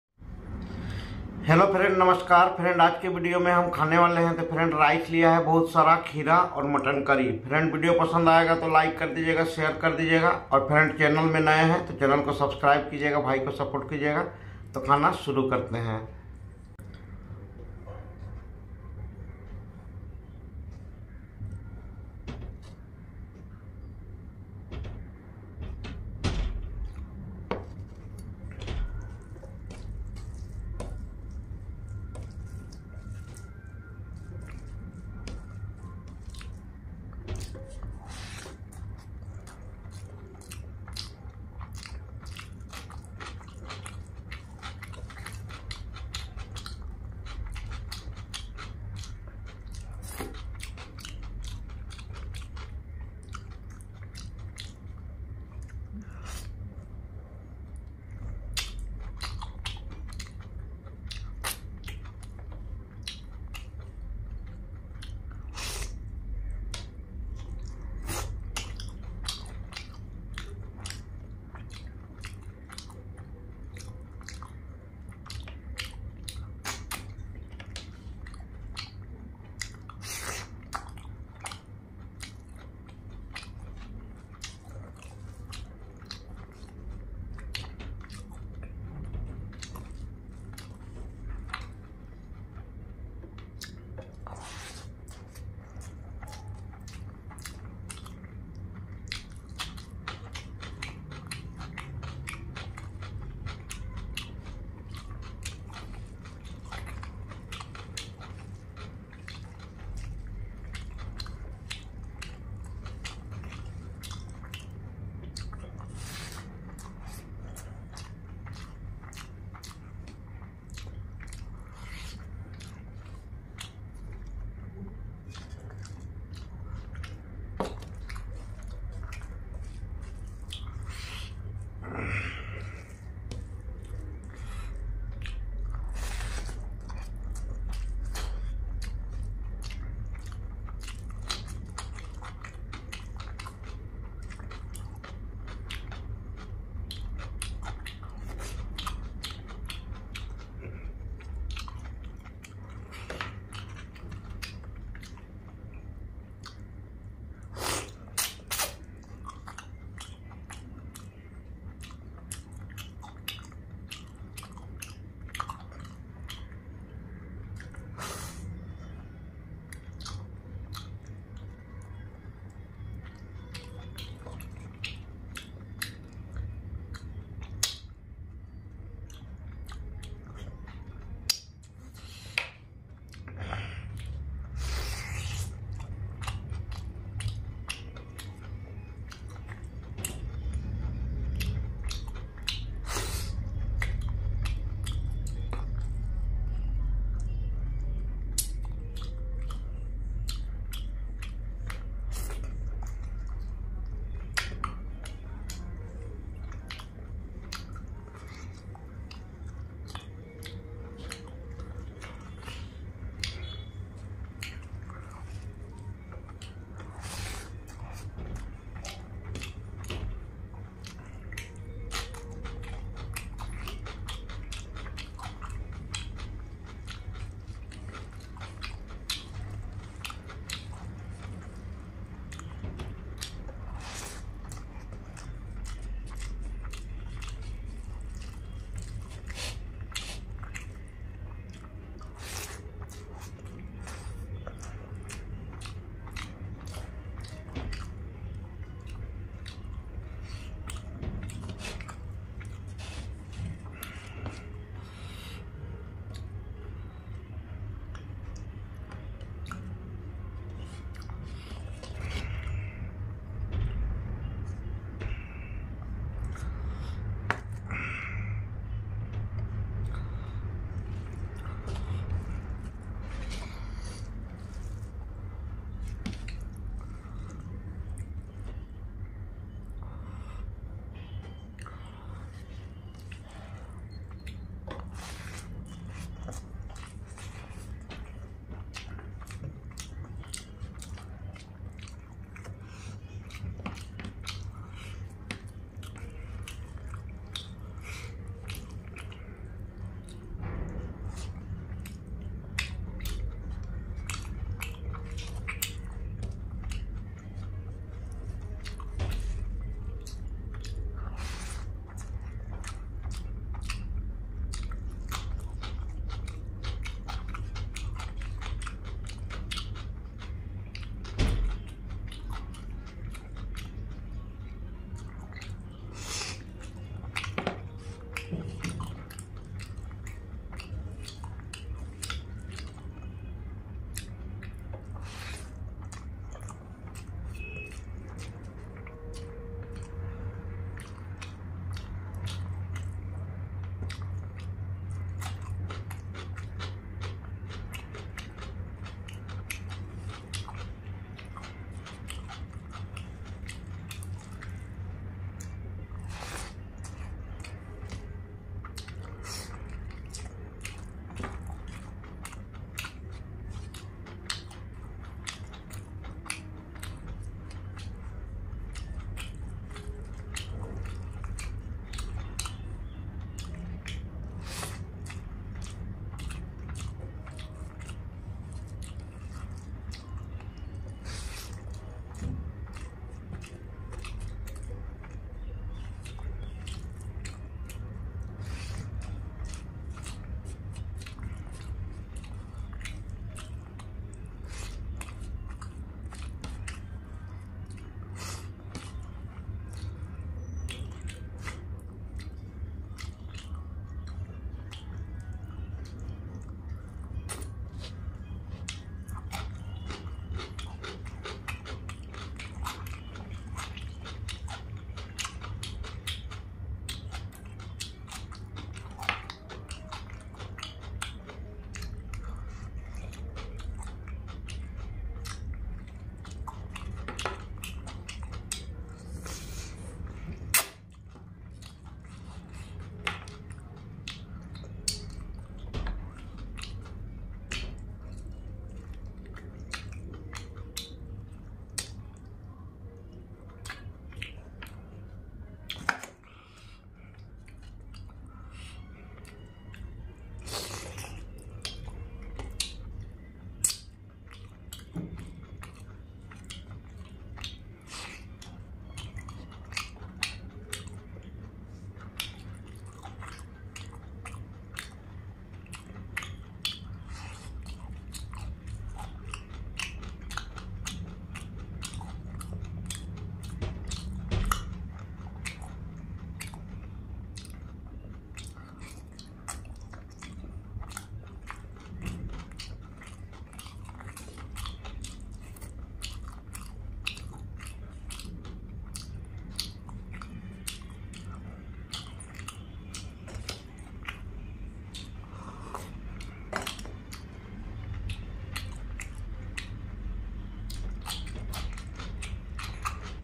ASMR EATING MUTTON CURRY RICE | BANGALI EATING SHOW MUKBANG EATING SHOW
HELLO GUYS IN THIS VIDEO I WILL SHOW YOU A SIMPLE  EATING SHOW. IT WILL BE LUNCH EATING. ITS AN NONVEG EATING SHOW AND BENGALI FOOD EATING
Eating show | Bangali eating show | Mukbang | Eating challenge | ASMR | Eating show Indian